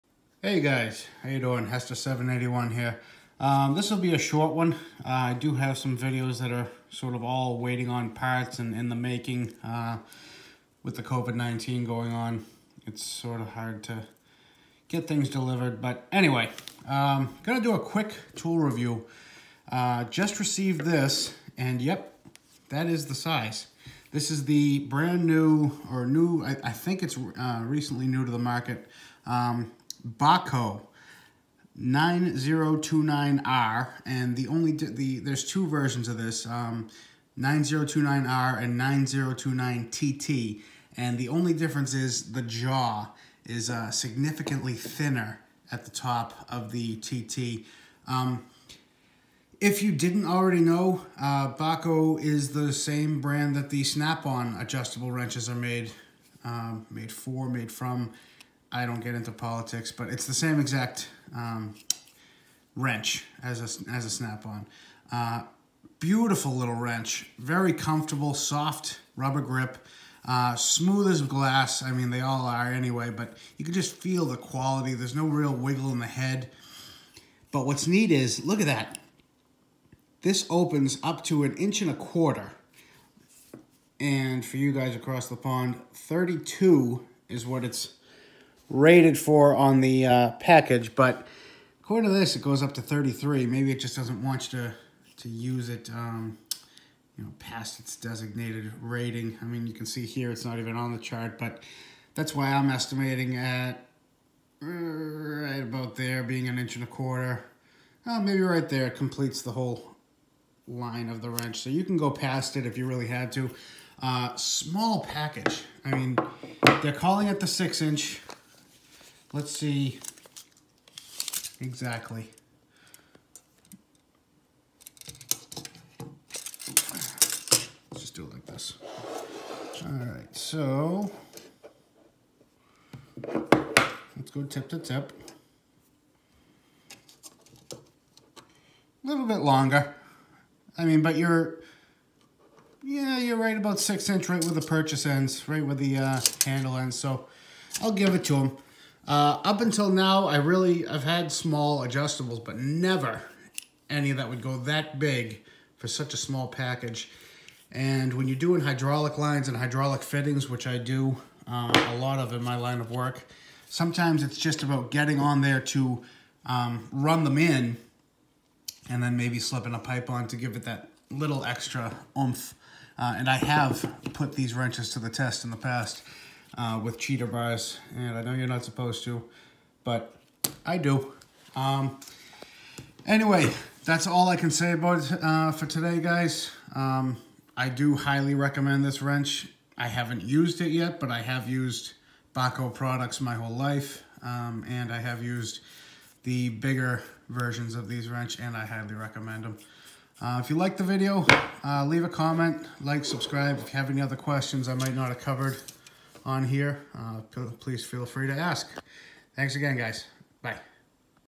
Bahco 9029R Adjustable Wrench Review
“Unboxing” and initial impressions of this beautiful wrench